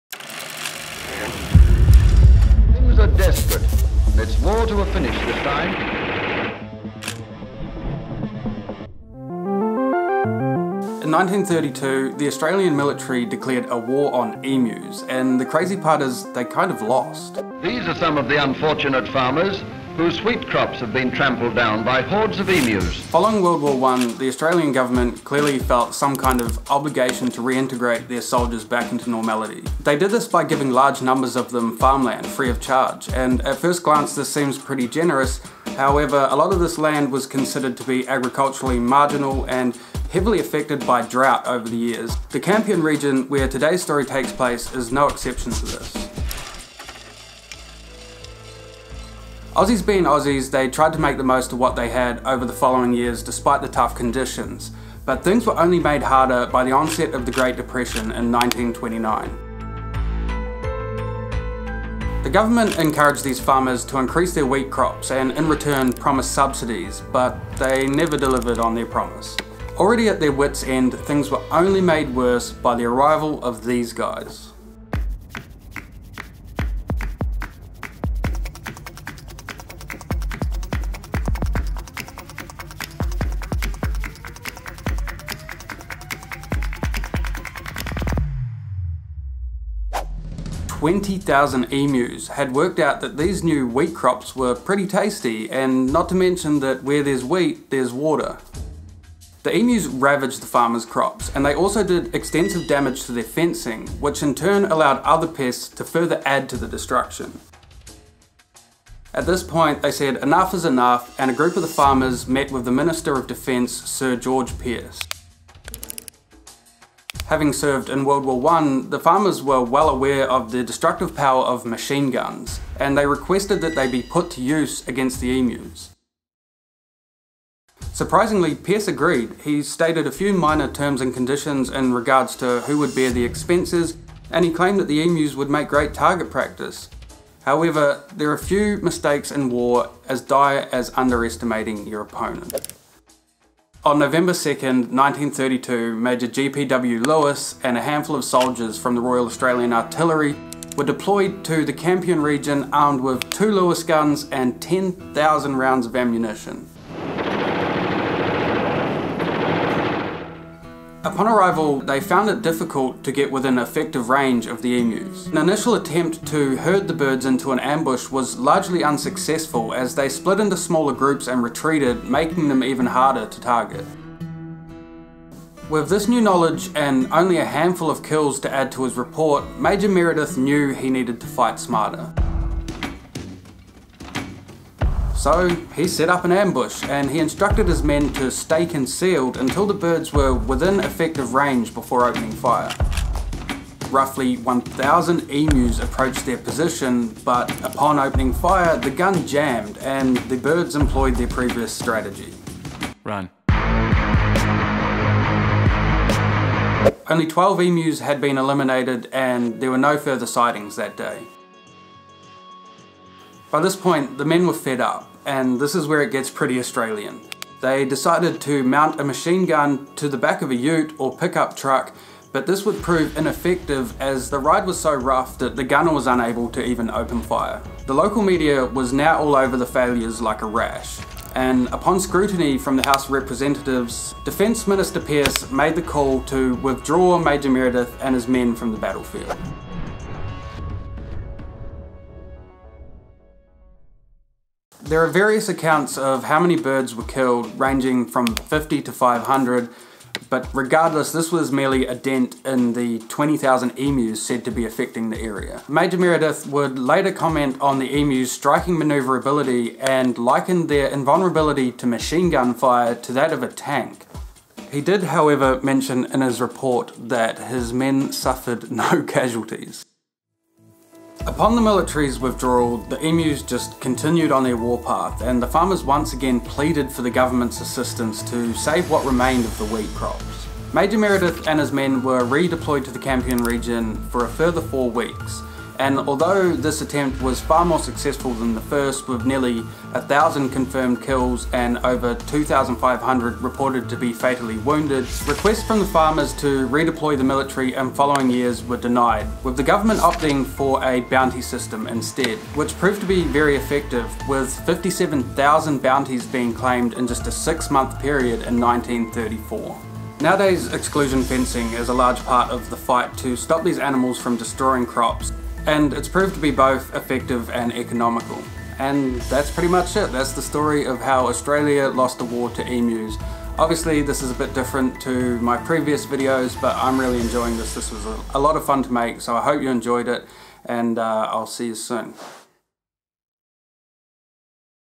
Why Australia Declared War On Emus | Great Emu War
This short documentary takes a look at how emus defeated the Australian military, this is definitely the most Australian thing I have heard in a while.
The Great Emu War took place in the Campion region of Western Australia in 1932 when former WW1 soldiers who re-settled as farmers pleaded with the Government for assistance in dealing with the growing problem of Emus destroying their wheat crops.
The Royal Australian Artillery came to their rescue and it gets pretty weird from their.